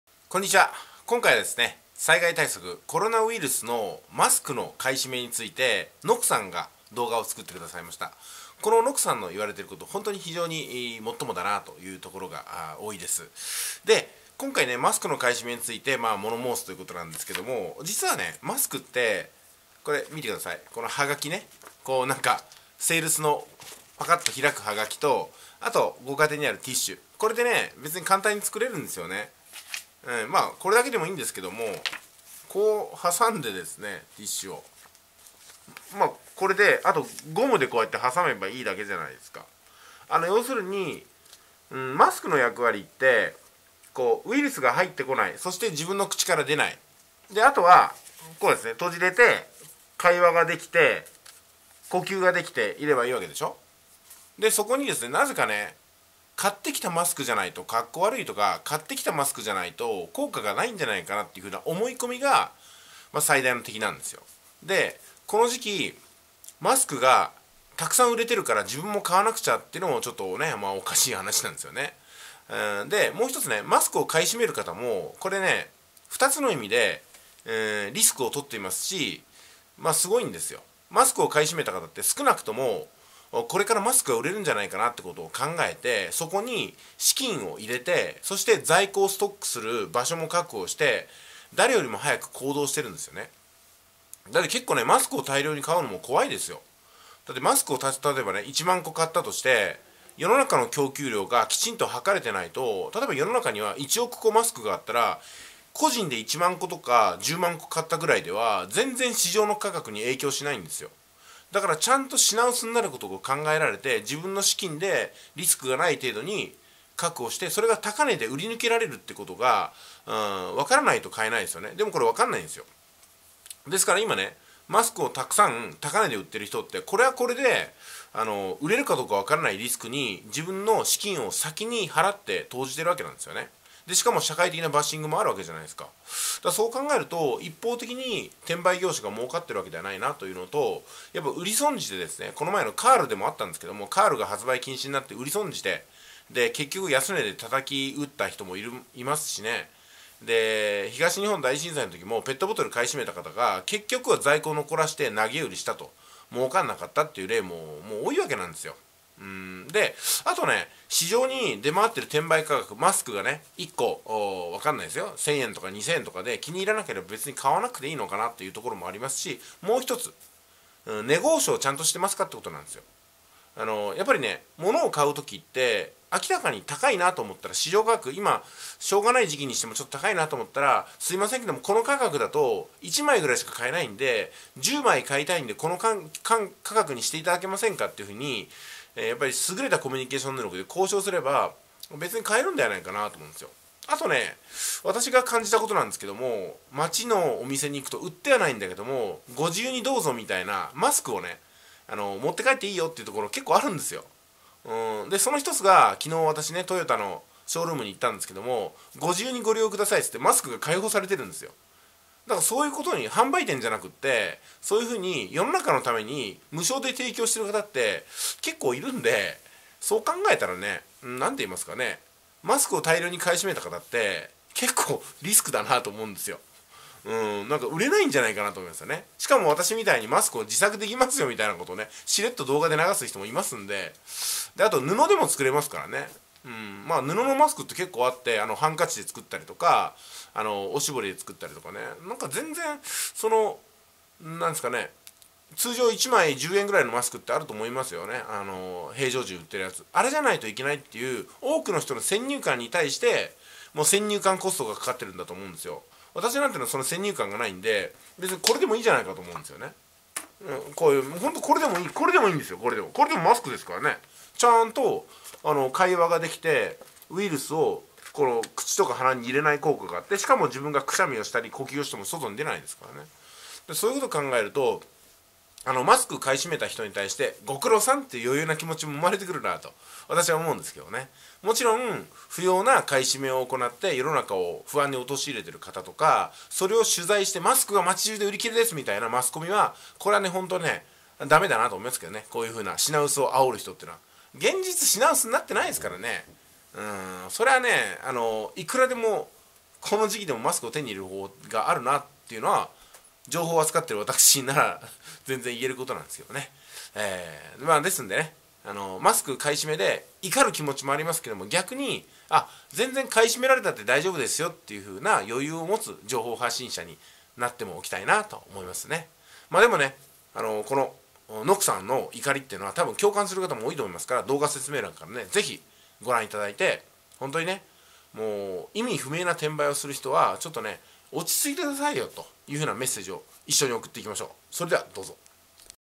【隠居TV】転売屋！マスク買い占めてんじゃねぇよ！
「NOKUさん」
最近のコロナウイルスの動画を私も作ってみました。

2020年3月末までの従来版「速稼塾」